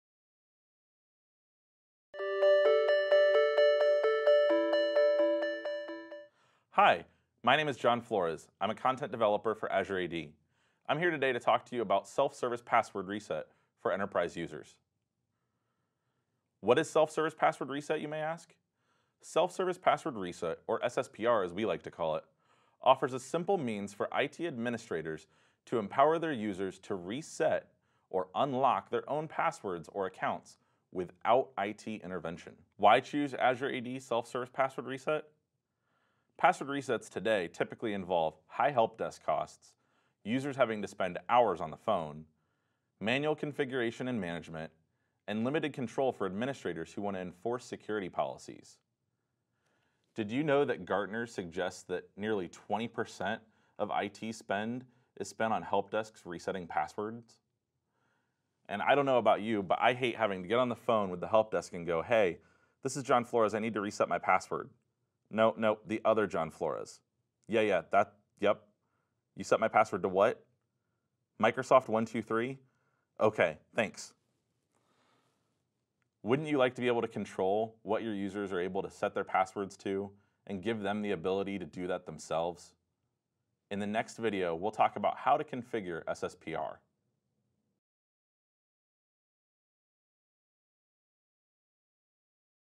What is self-service password reset? | Microsoft Entra ID
Self-service password reset offers a simple means for IT administrators to empower their users to reset or unlock their own passwords and accounts without IT conventions. This video will help educate IT administrators and end users on the self-service capability of resetting a forgotten or expired password in Microsoft Entra ID.